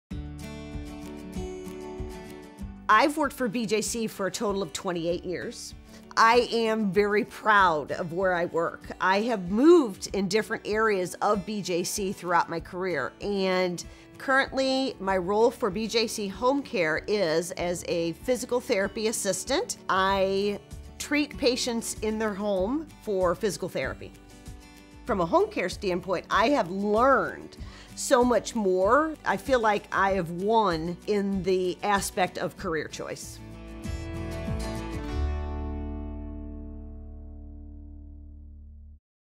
BJC Home Care: Meet Debbie Teeter
Debbie Teeter, PTA is a physical therapy assistant with BJC Home Care, the largest and most comprehensive home care provider in the St. Louis region, serving thousands of patients in more than 25 counties in Missouri and Illinois.